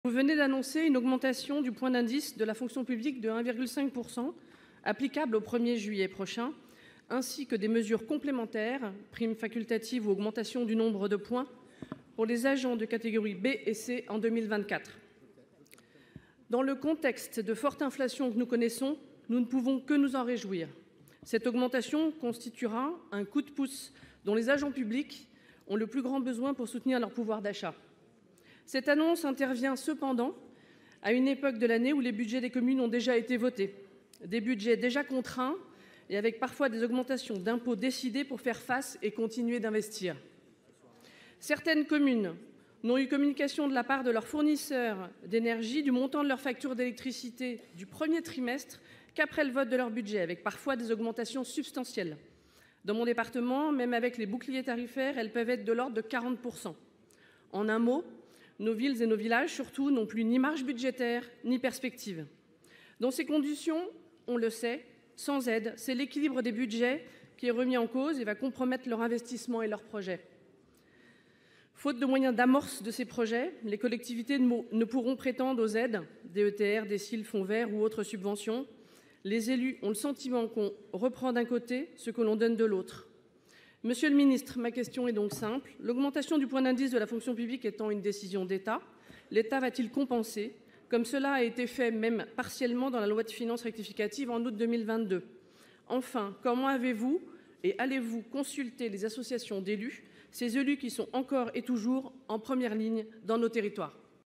Un coup de pouce dans le contexte de forte inflation | Daphné Ract-Madoux
Daphné Ract-Madoux, Sénatrice de l'Essonne, est intervenue en séance de Questions au Gouvernement au Sénat ce mercredi 14 juin, pour interroger Stanislas Guérini, Ministre de la Transformation et de la Fonction publiques de France, sur l'augmentation du point d'indice des fonctionnaires.